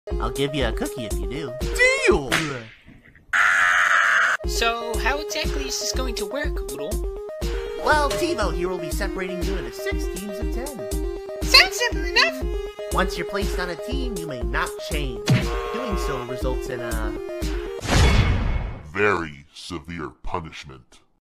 Animated Inanimate Battle M.A.P Part 18
This Is My First Map I’ve Competed In,I Love AIB So I Decided To Compete In This M.A.P. Here Is The Link To The Map